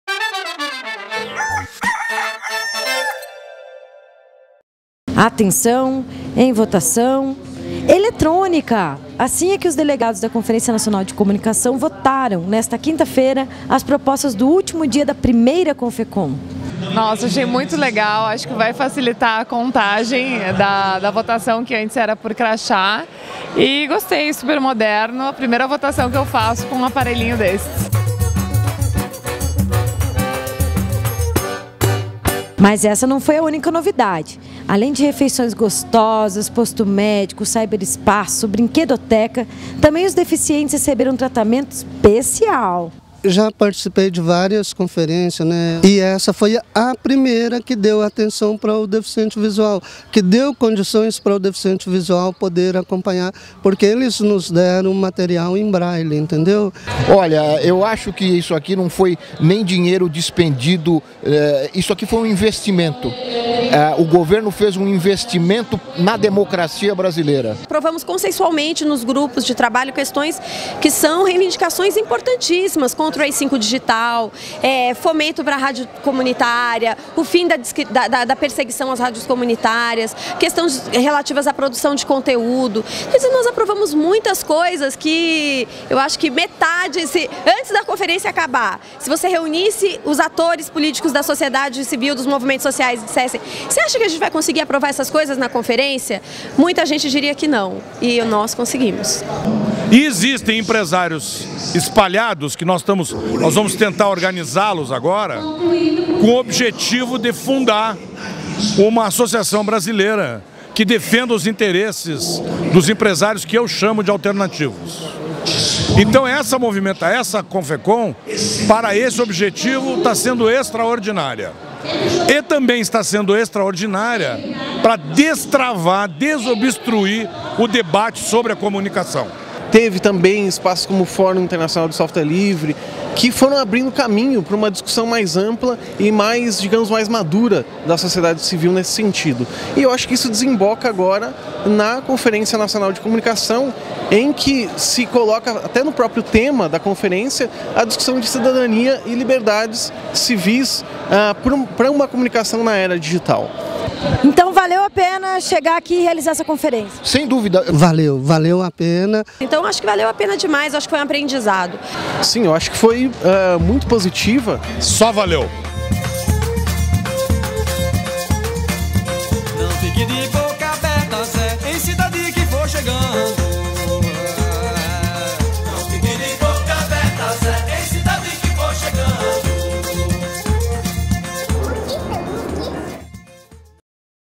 Os três setores se supreendem com o resultado da Confecom
A Primeira Conferência Nacional de Comunicação é um marco na democracia brasileira. Está é a avaliação de Laurindo Lalo Leal Filho, ouvidor da EBC,
Joaquim Ernesto Palhardes - Diretor da Agência Carta Maior e da Renata Mielli do Portal Vermelho. Os três setores viram muito progresso no diálogo em torno da comunicação pública no país. Veja o vídeo da TV Vermelho com o entusiasmo do último dia da Confecom.